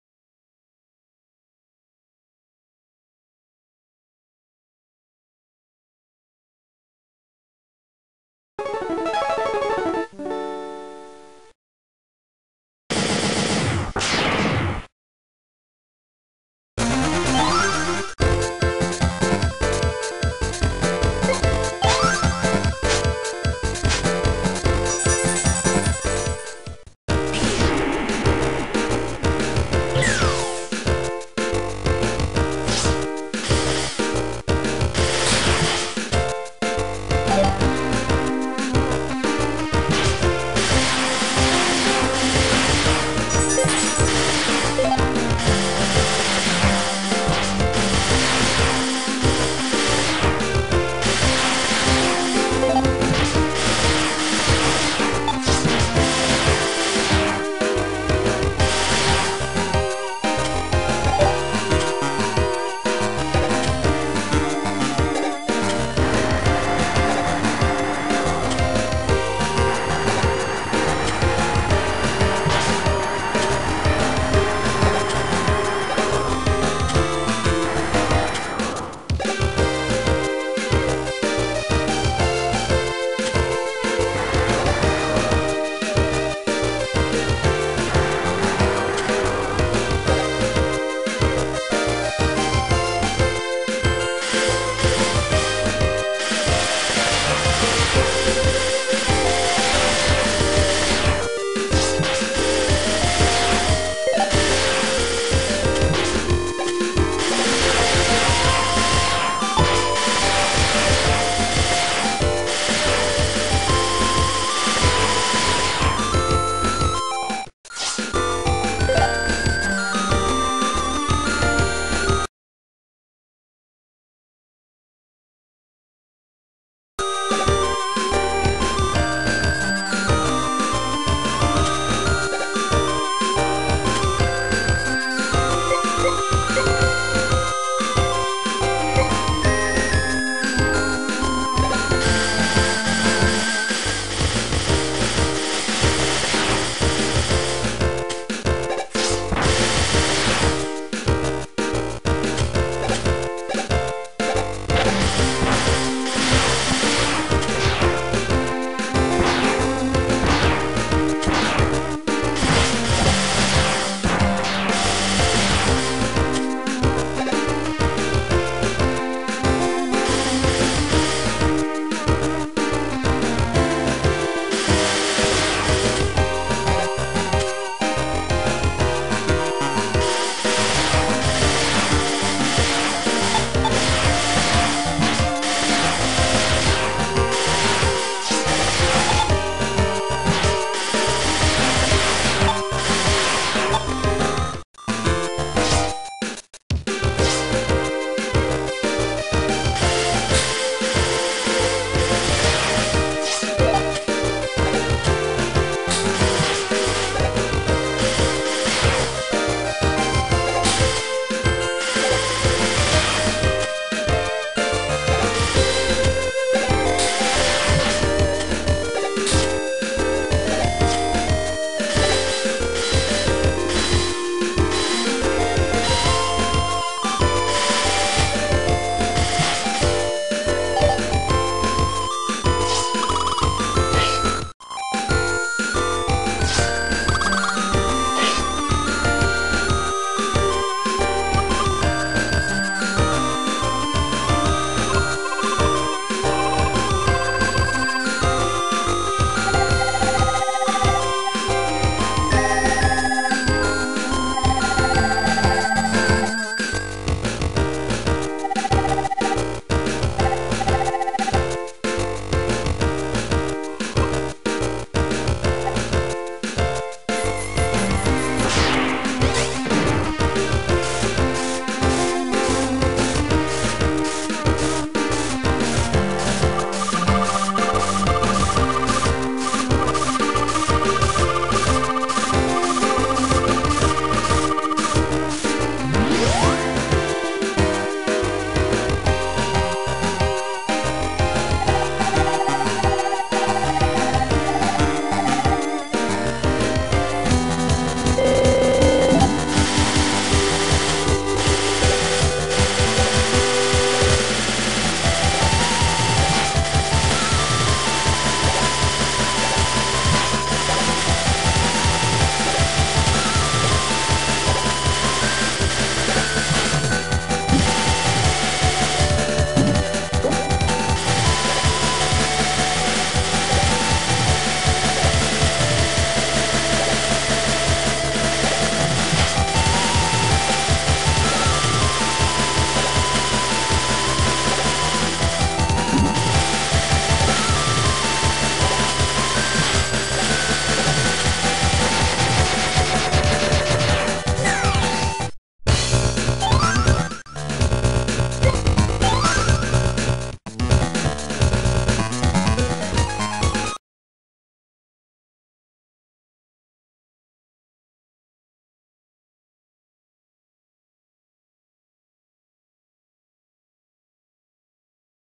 bass walkthrough
yahoo. time for bass to take the stage. why am i doing both of them? one it's fun to see how different i play in the same stage. two once, i'm done with them i can do some digimon battle spirits or get a new rom to walkthrough. enjoy me killing.

comment and rat me please